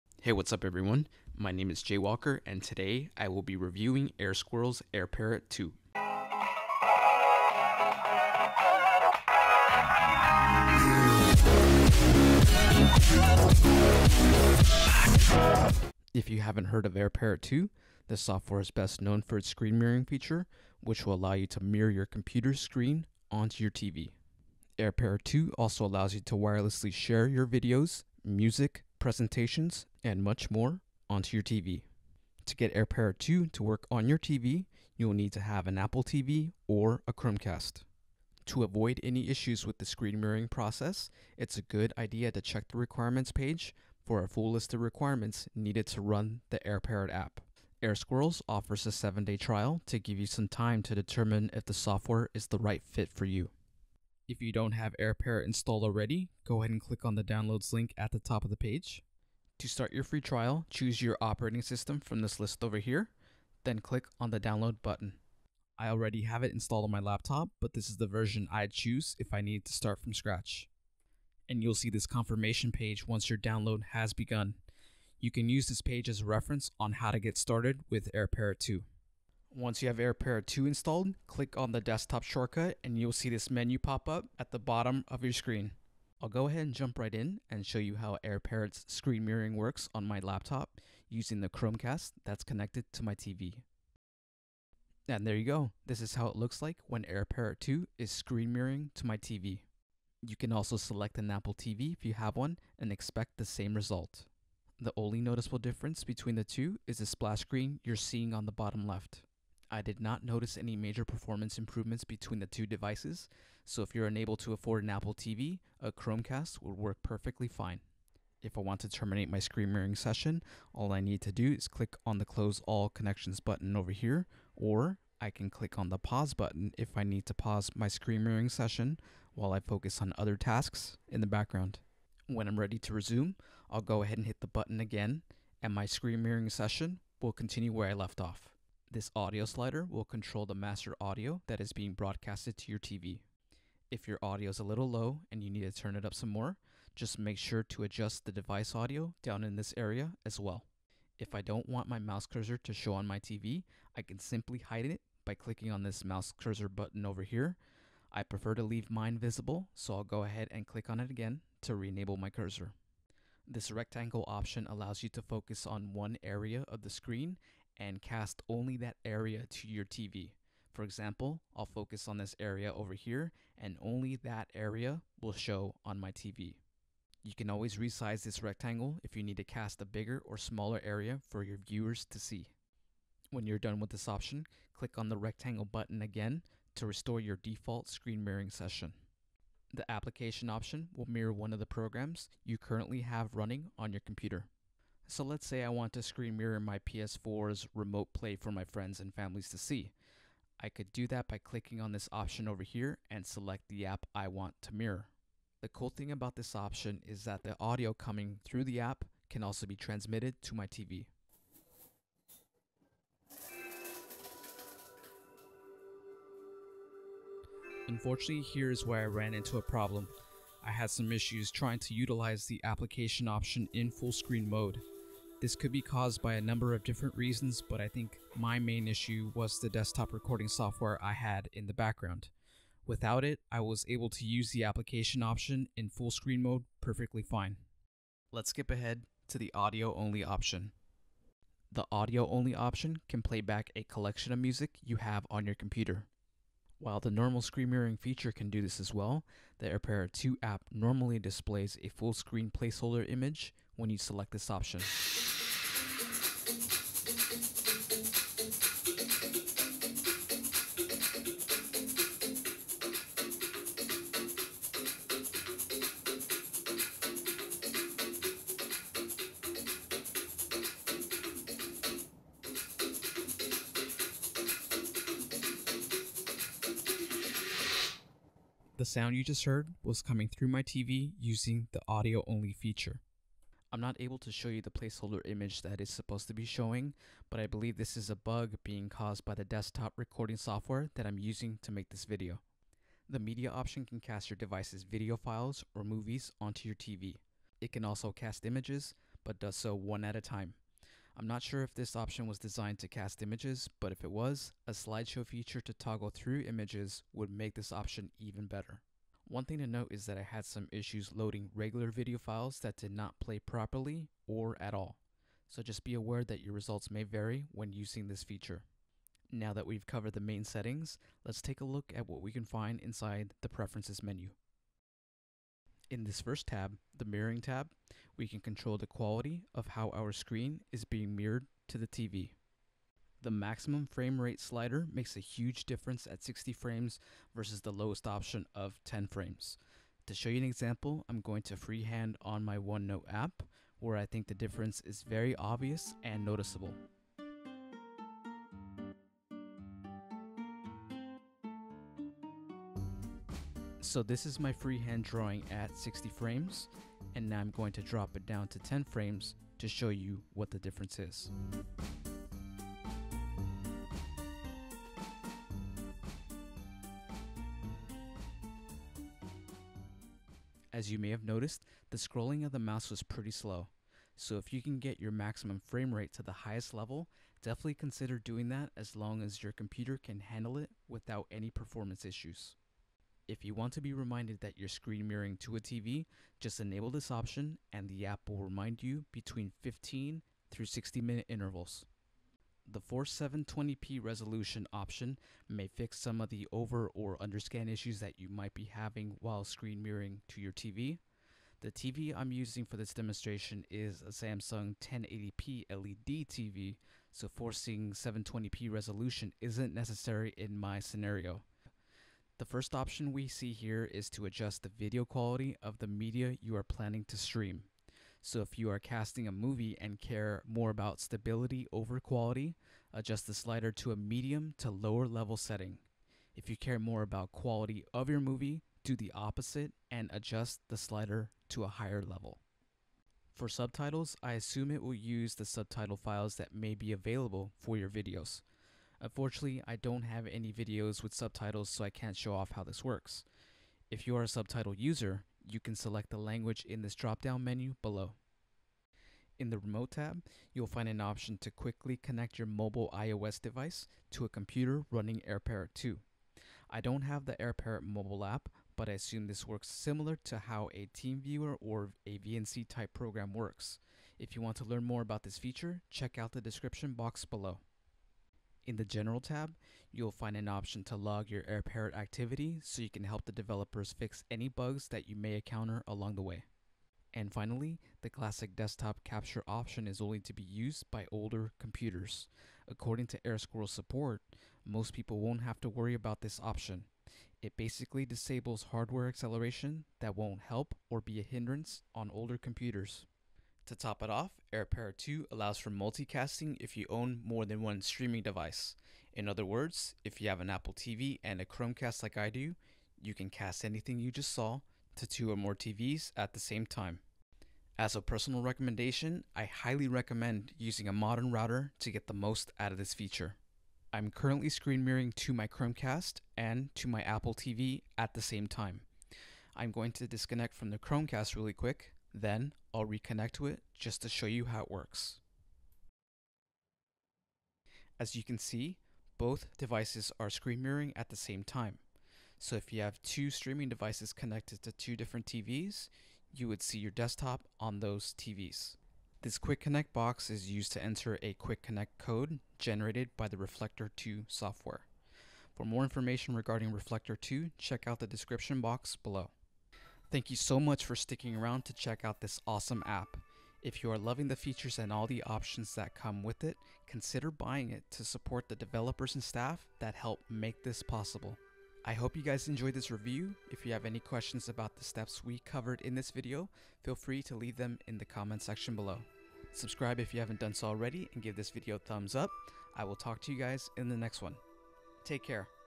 AirParrot 2 Review | Screen Mirroring with a Chromecast & Apple TV
This video will break down Squirrels AirParrot 2 and review the app's best features. AirParrot is able to screen mirror onto one or multiple TV's at the same time. Users of all experience levels will find the app feature-rich and easy to use. The laptop I used in this video was running the Windows 10 (64-bit) operating system. AirParrot is also available for Mac and Chromebooks.

►Additional Info

Fire TV & Fire TV Stick users:
Screen mirroring to a TV is possible. You will need to have AirParrot *AND* Reflector 2. You can see it in action here:

PS4 Users:
The Playstation 4 "Remote Play" app can be screen mirrored to another TV without issues, but you may experience a little lag in the process.

Kodi users:
I attempted to screen mirror using Kodi's Airplay hotspot without any luck. Kodi's Airplay feature is a work in progress which hopefully gets updated soon.

Anything is appreciated. Thank you!